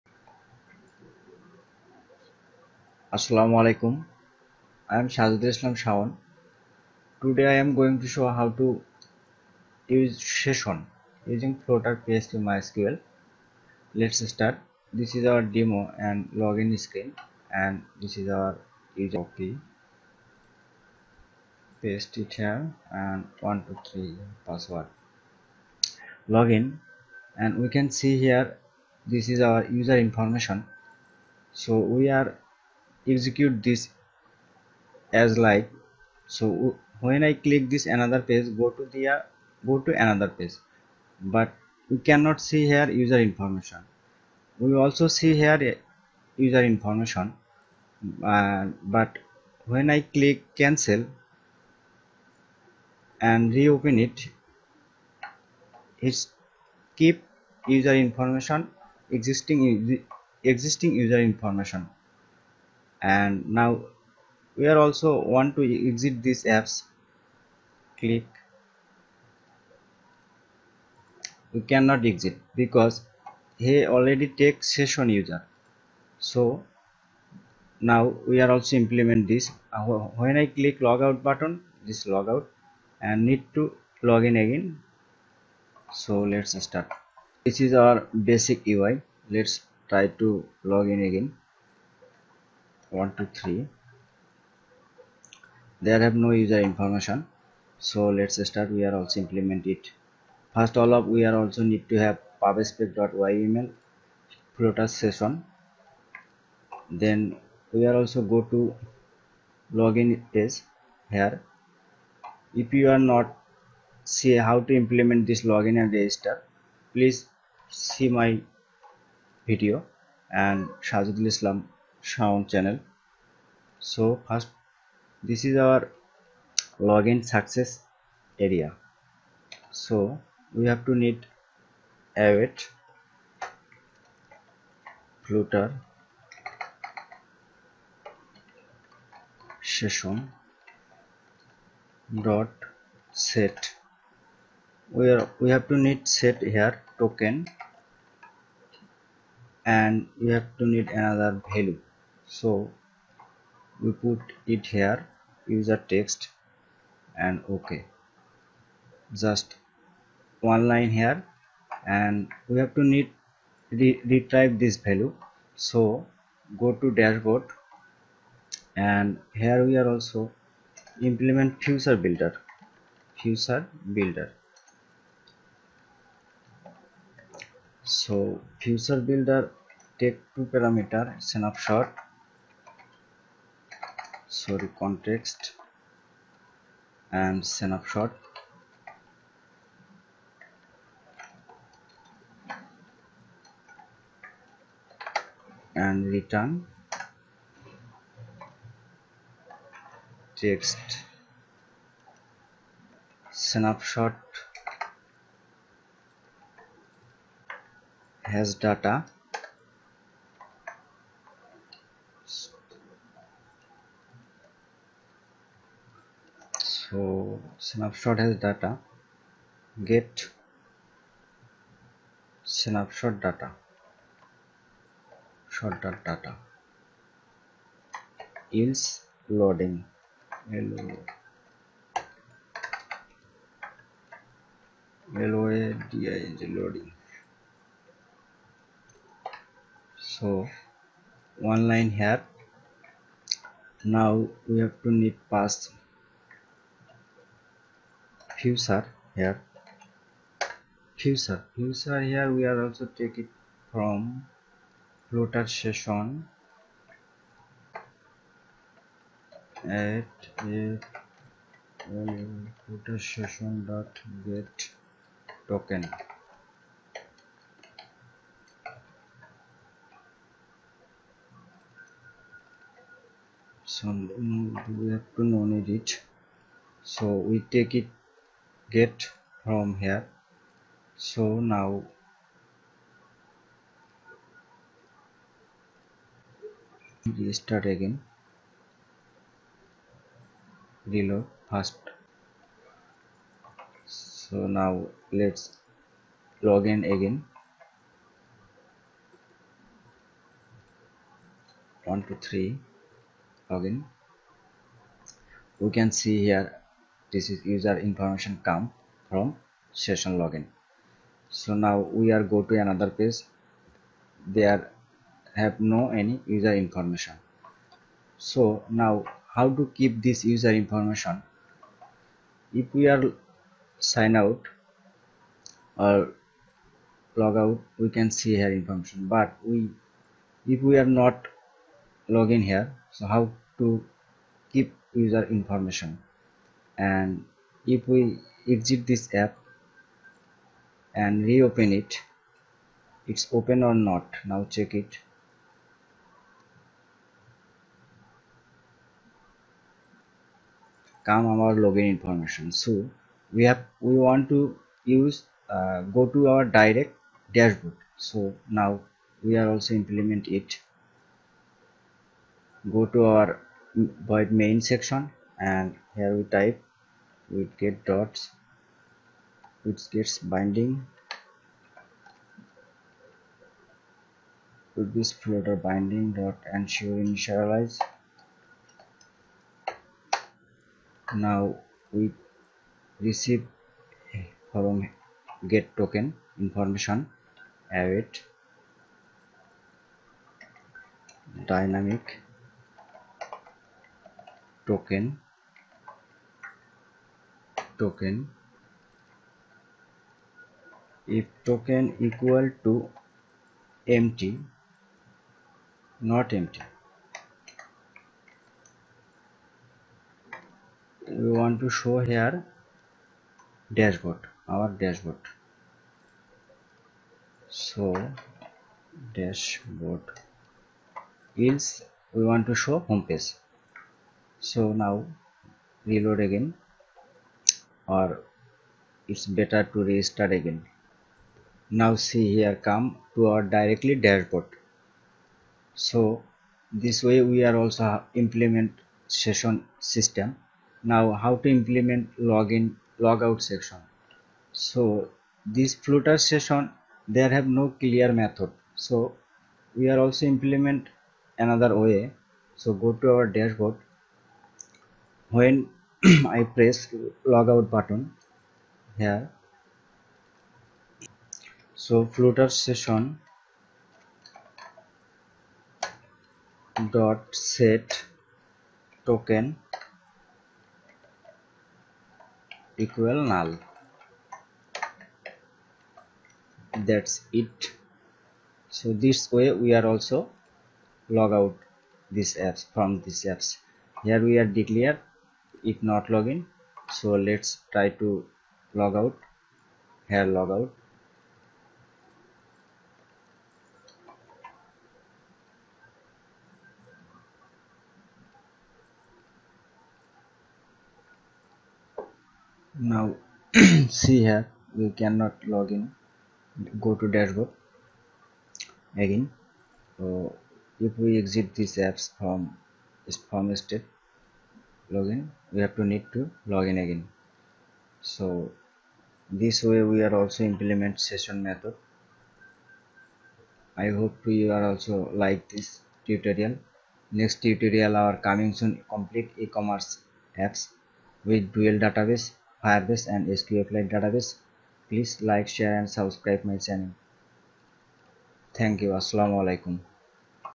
Flutter Session login and keep logged in
Flutter Session login and keep logged in with user information.